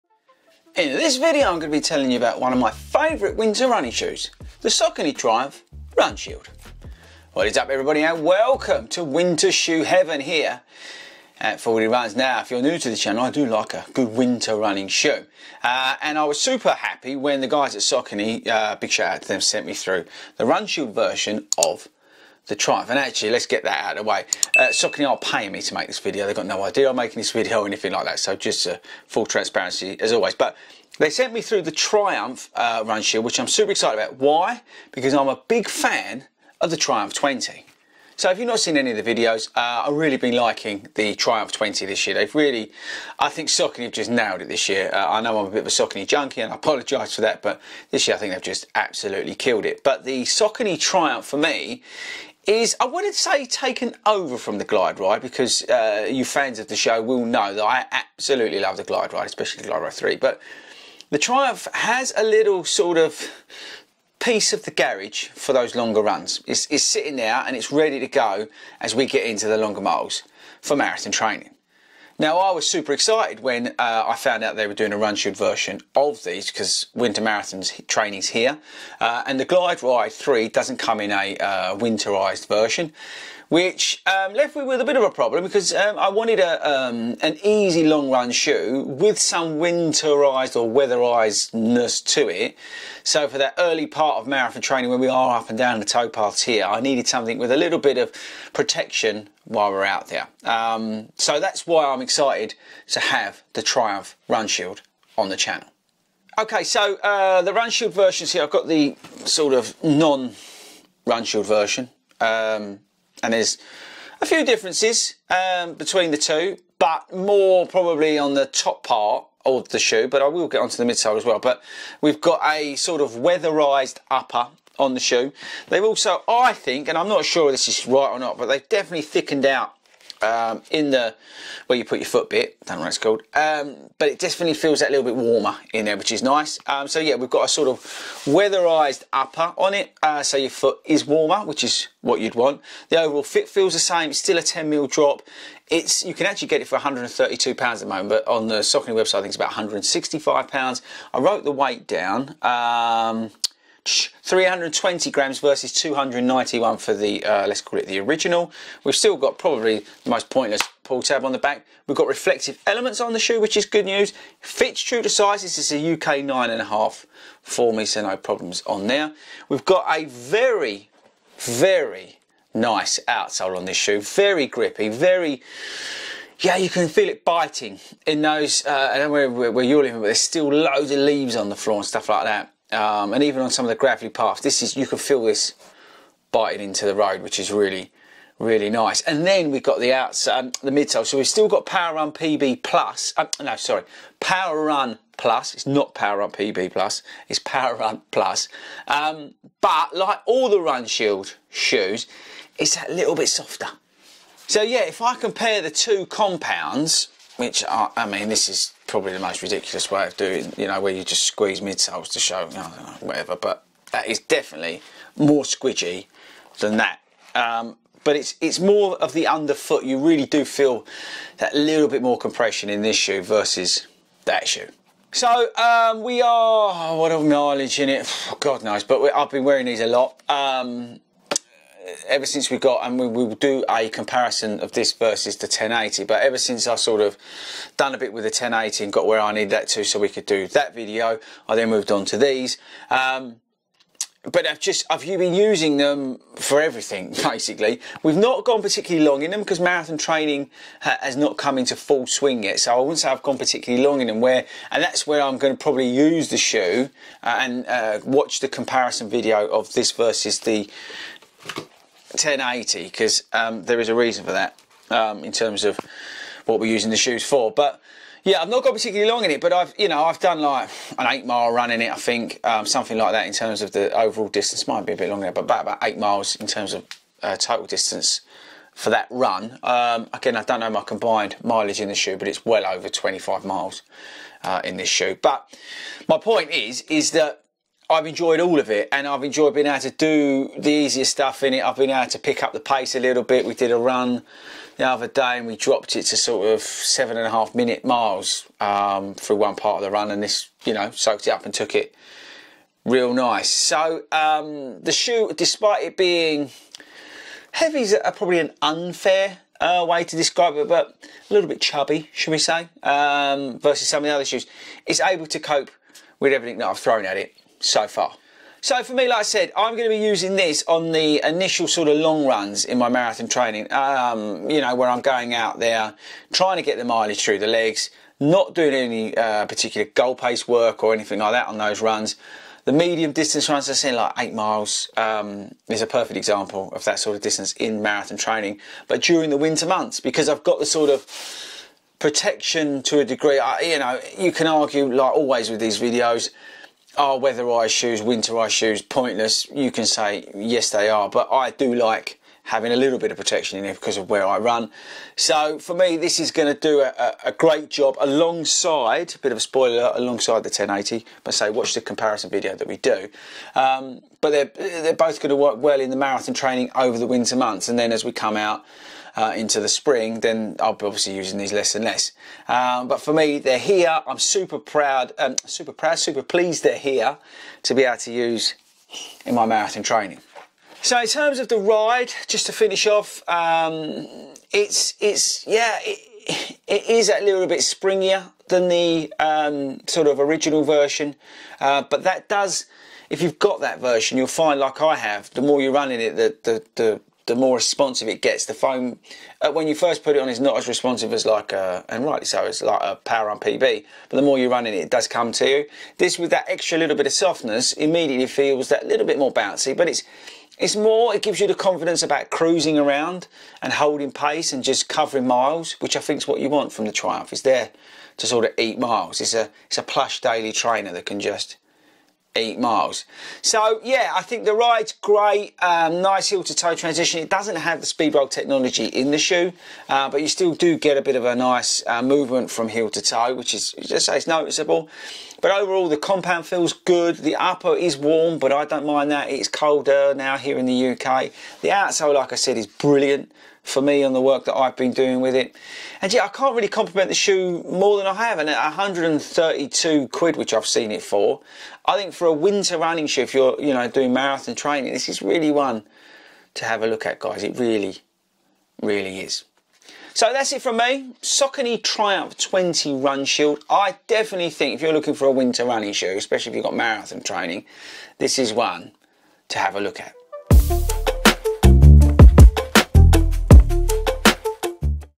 Saucony Truimph 20 Runshield Review
Saucony Truimph 20 Runshield Review! What is the Runshield version of the popular daily trainer from Saucony like? The Truimph 20 has received a winter weatherised pack to keep your feet warm during winter marathon training! Is this the best winter running shoe?

TO DO'S!